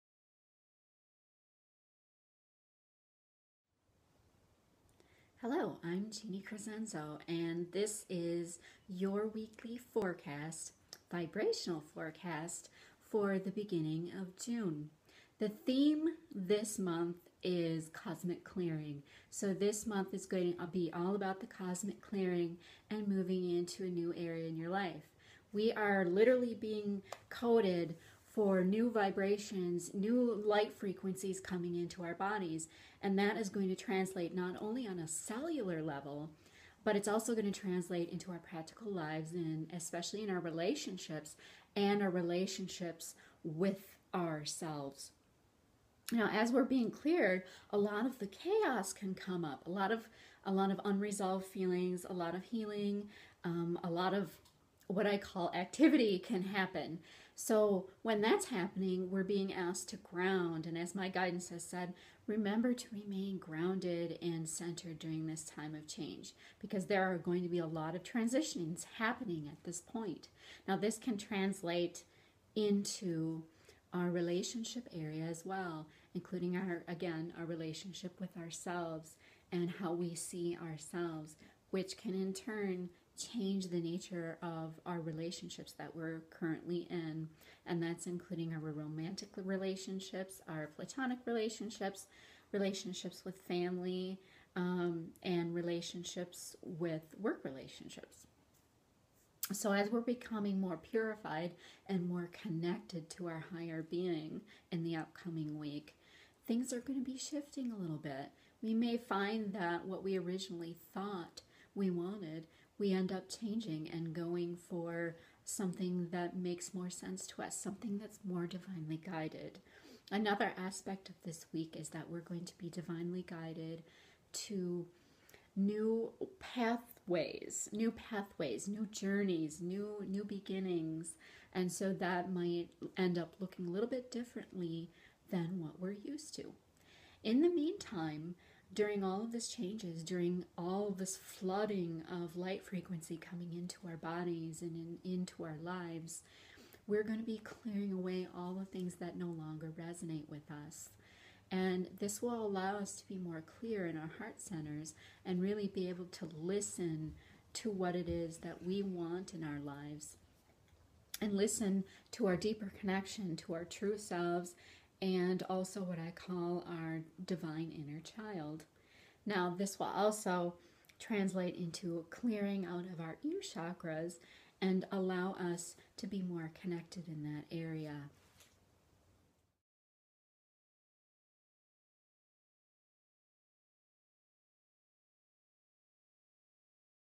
Galactic Vibrational Forecast June 1st-7th
Energetic forecasts for the week that support your spiritual growth and wellness.